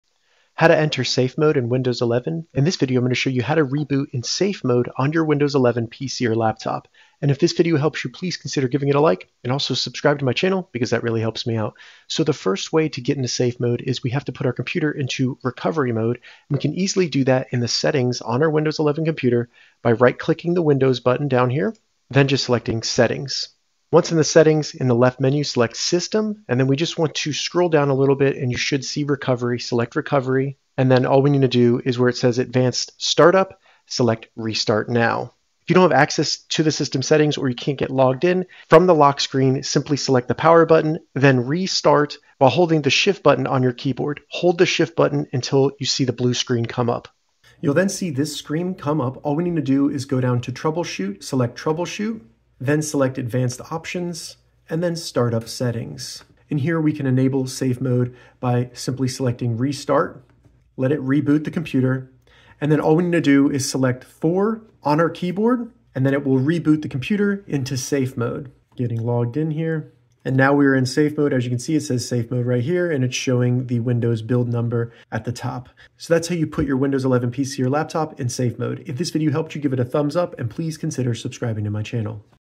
How to Enter Safe Mode on Windows 11 PC & Laptop
How to Enter Safe Mode on Windows 11 PC & Laptop? In this tutorial, I show you how to boot your Windows 11 computer into Safe Mode using multiple methods including Shift + Restart & recovery mode through the Windows 11 settings. This means you’ll be able to troubleshoot and fix issues like crashes, viruses, or startup problems in safe mode Windows environment.